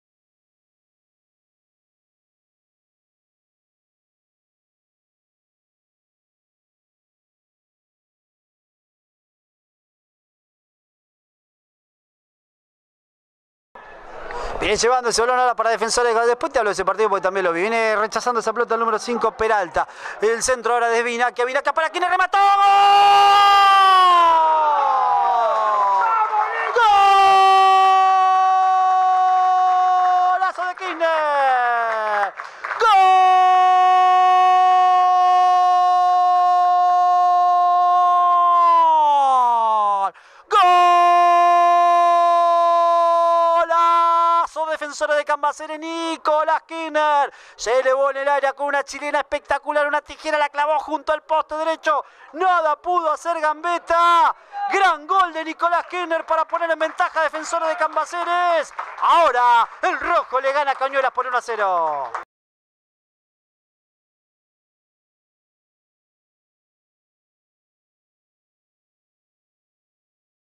GOLAZO DE KISSNER
Gran tanto del delantero de Camba en un encuentro ante Cañuelas jugado en la cancha de Argentino de Quilmes en 2015.